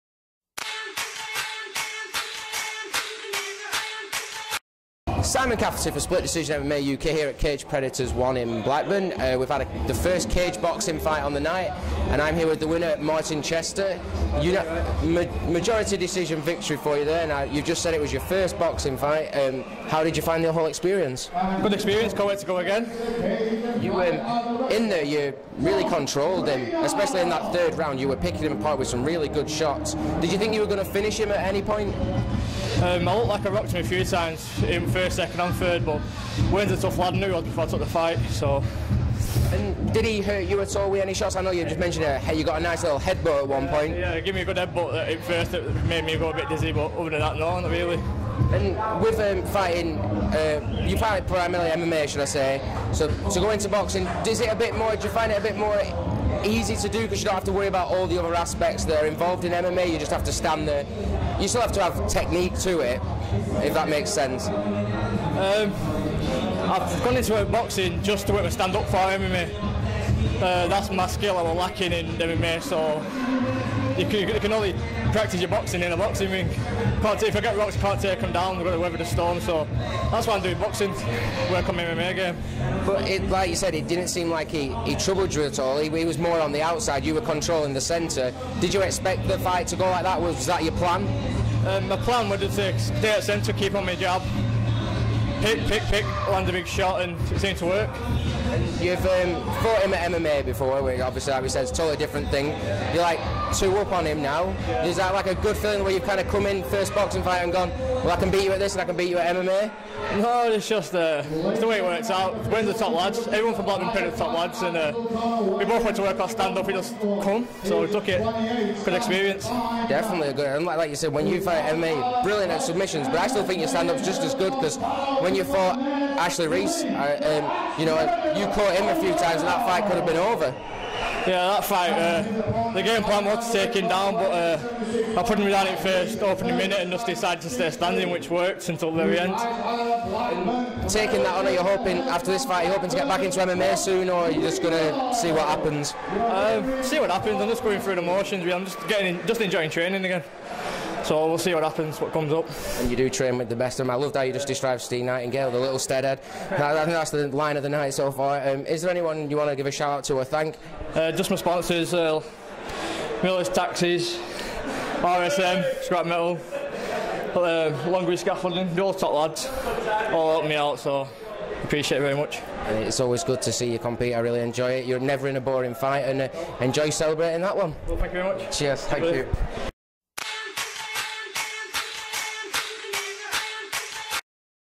Caged Predators 1 - Post Fight Interview: Martin Chester
REPS MMA's Martin Chester is known for his Mixed Martial Arts skills inside the cage but last night he was involved in a caged boxing match which took home SD MMA UK's fight of the night.

He stood toe-to-toe with Blackburn Predators Wayne Tierney and came out on top via majority decision.

After the cracking match up, he joined me to talk about the fight.